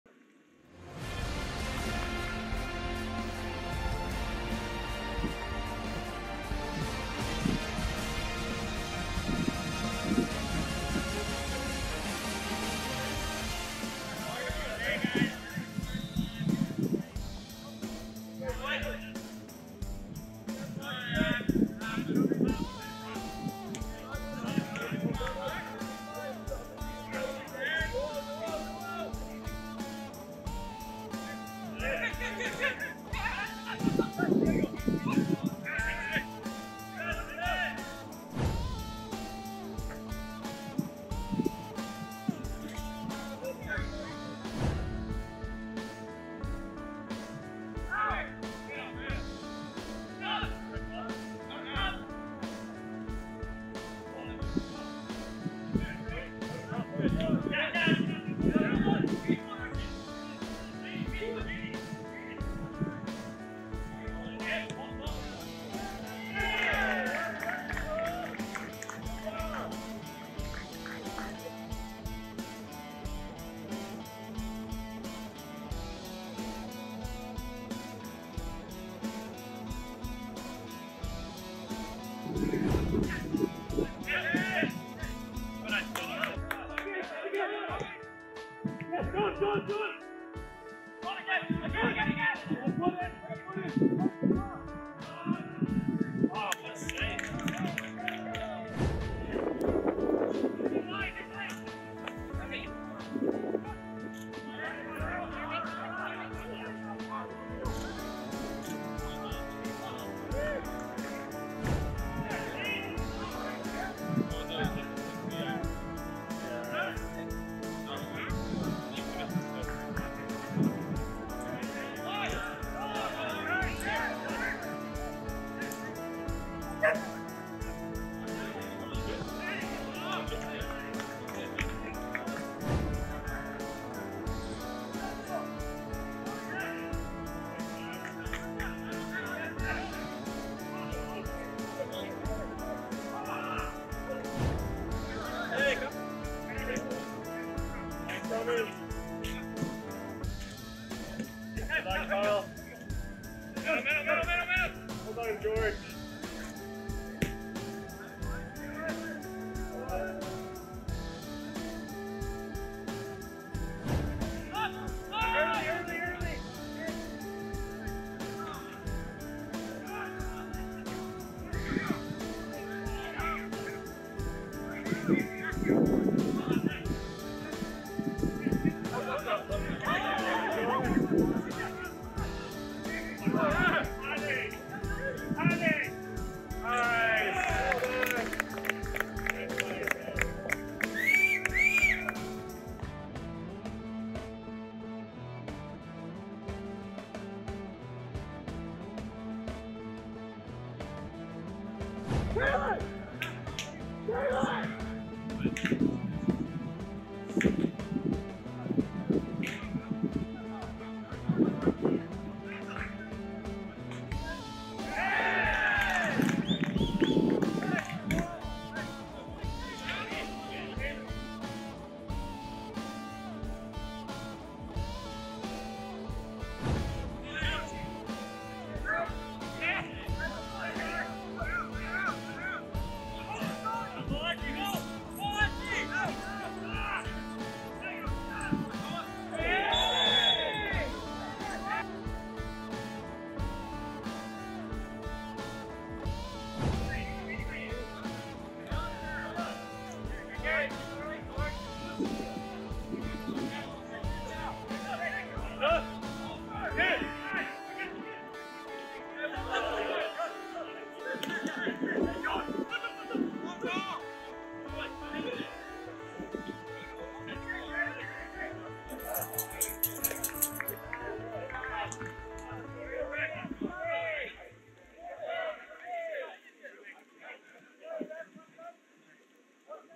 HIGHLIGHTS: Cook Street United vs Gorge FC
The best parts from this preseason friendly match played at GNS